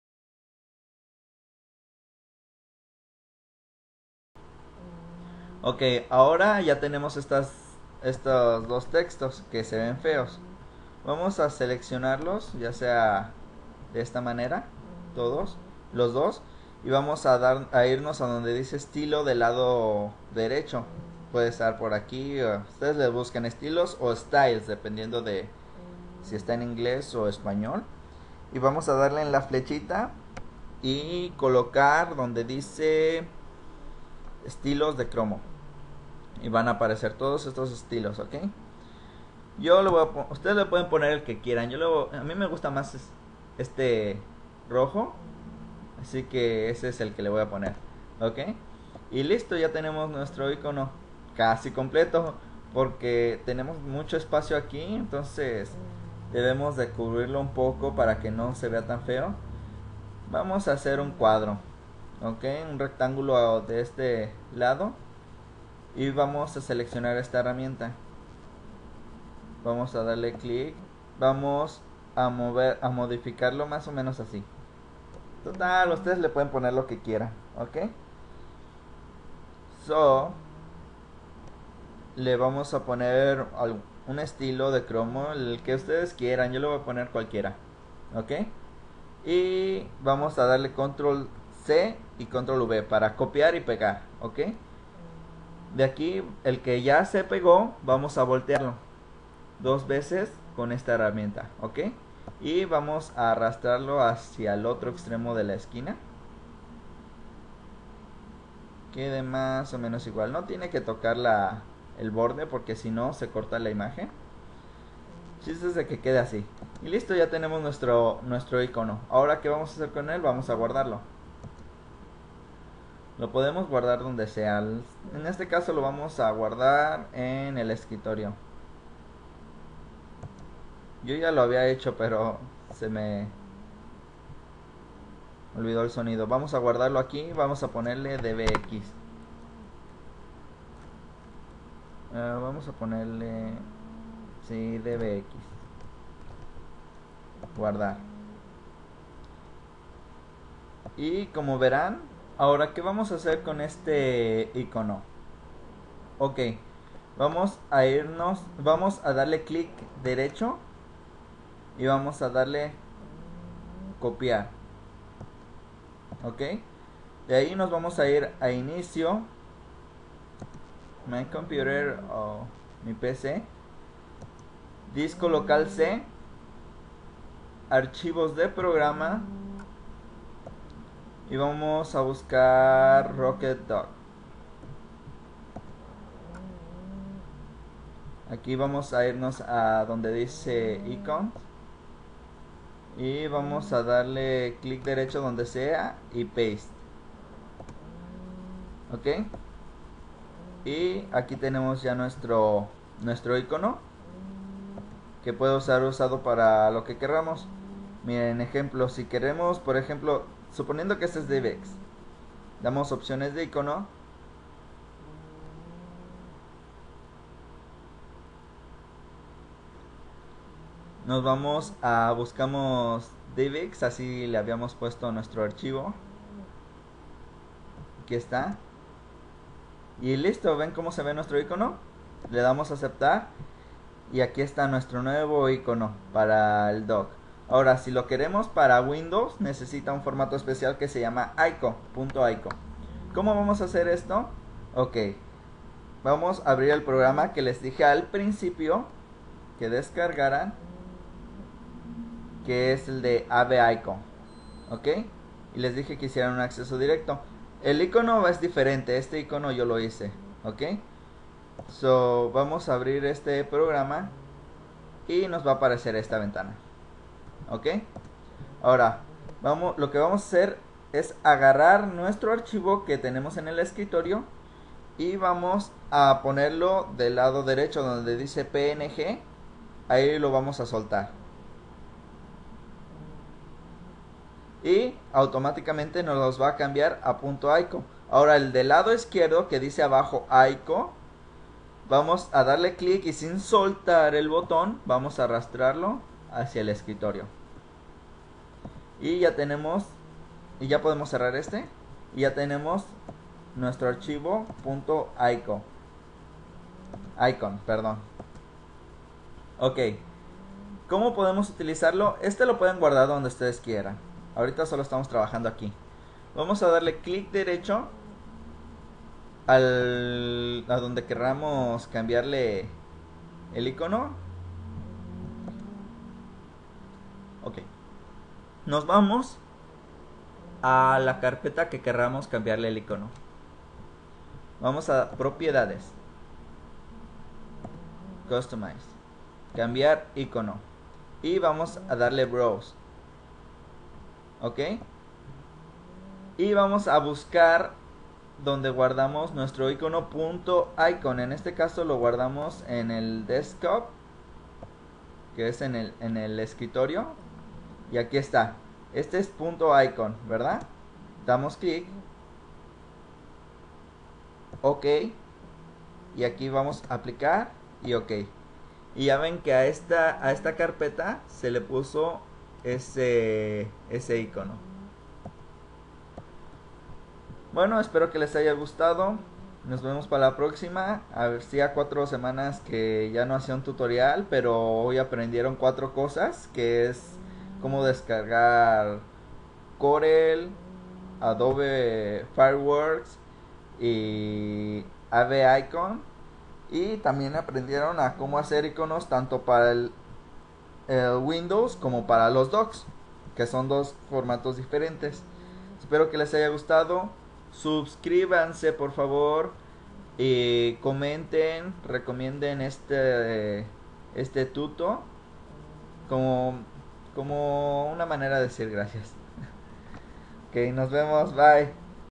como crear iconos parte (profecionales) 2/2
Esta es la segunda parte de:
Como crear ICONOS para docks  .PNG, y para Windows .ICON
sirbe para:objectdock, rocketdock, windows, etc